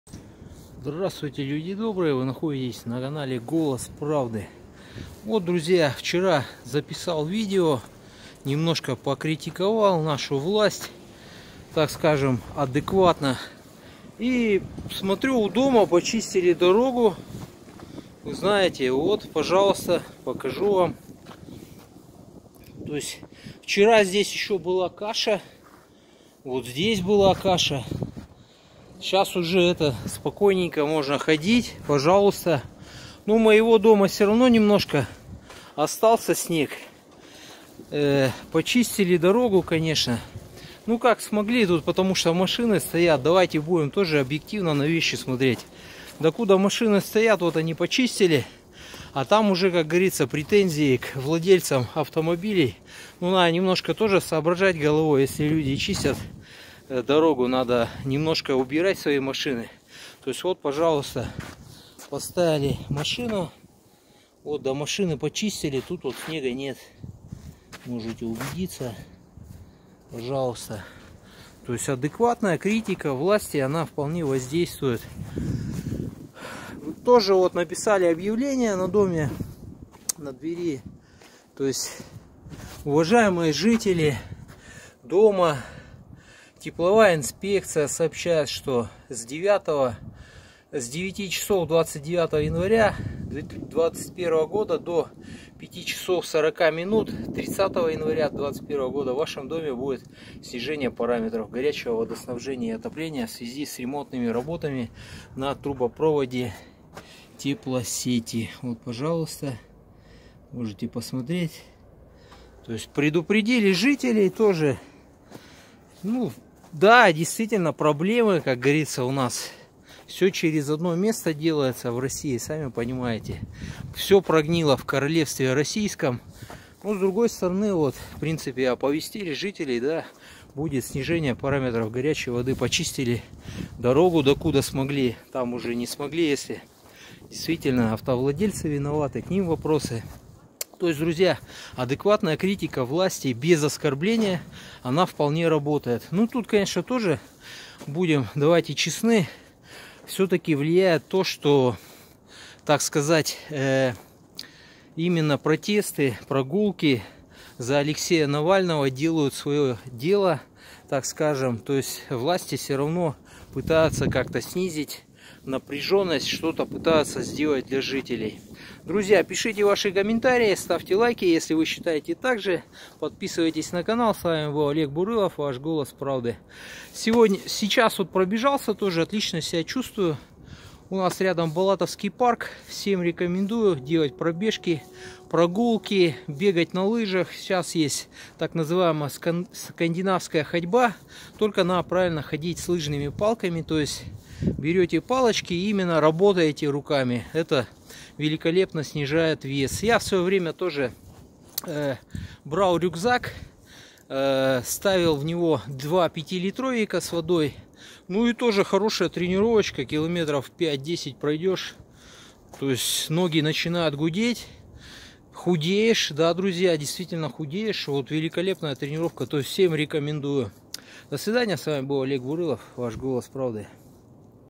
Конструктивная критика власти работает...после протестов. 😉 Занимайтесь физкультурой! 💪
Буквально через сутки после моего ролика был расчищен снег возле моего и соседнего дома. Конструктивная критика без оскорблений работает! К нам начинают прислушиваться! Занимайтесь физкультурой и побольше гуляйте на свежем воздухе.